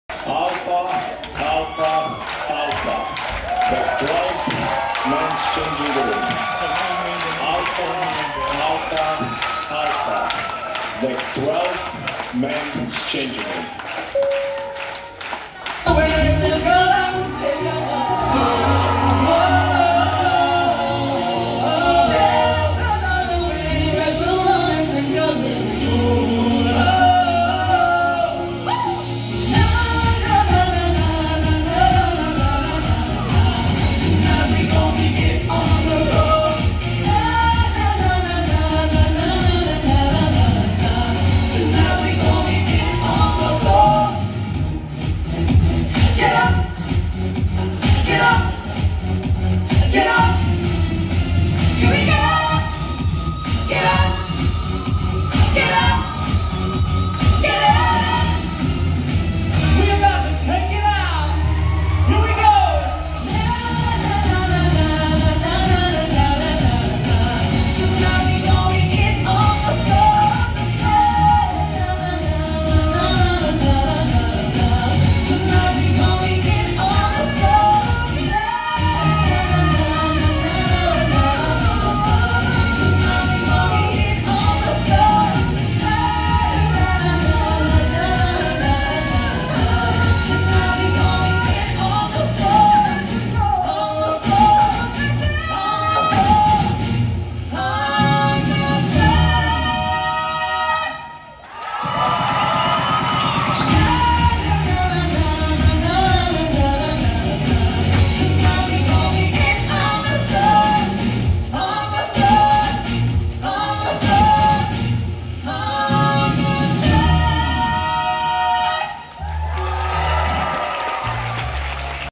Cruise Ship ALPHA ALPHA ALPHA Emergency but RCCL Show Kept Going
Video taken on RCCL Navigator of the Seas in Nov 2023.  It was impressive to see the performers keep going despite losing all music and be in the perfect place when the music came back!